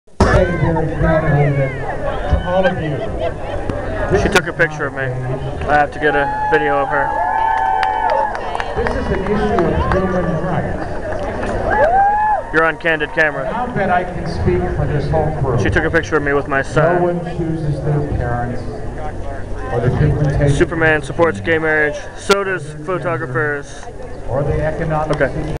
Preparing for the Largest Gay Wedding in the US
2012 in Cleveland, OH